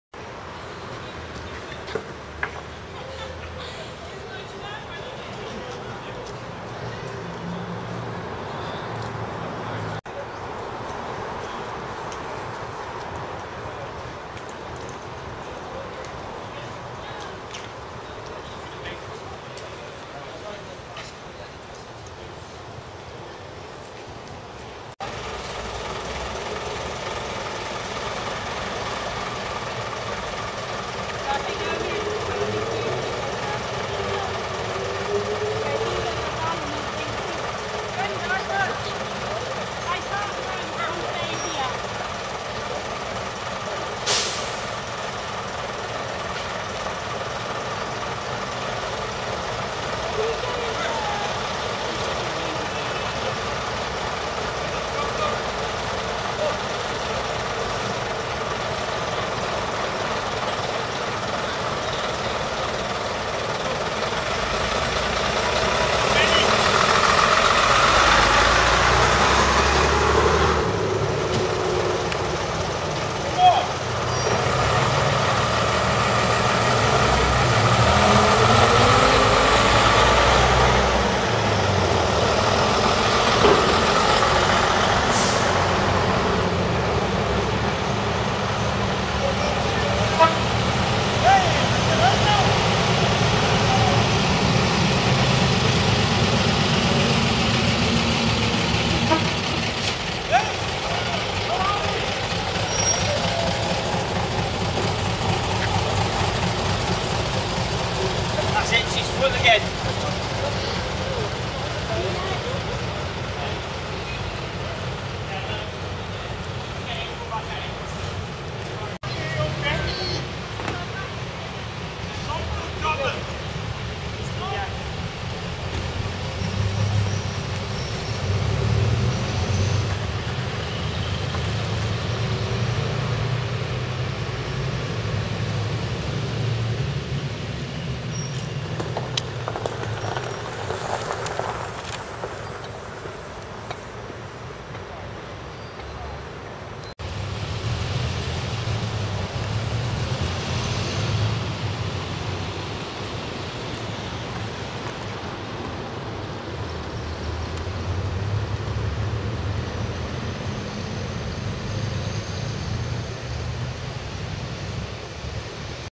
STAGECOACH WEST RUNNING DAY 310514-ROUTE 94 CONVOY ON LAST TRIP OF DAY
At the end of the Stagecoach West Running Day ,MD Ian says his goodbyes and a convoy of heritage vehicles depart for Cheltenham on Route 94 ,seen are MFN946F,JAH553D,WLT835,SMK760F,841SHW-Not in order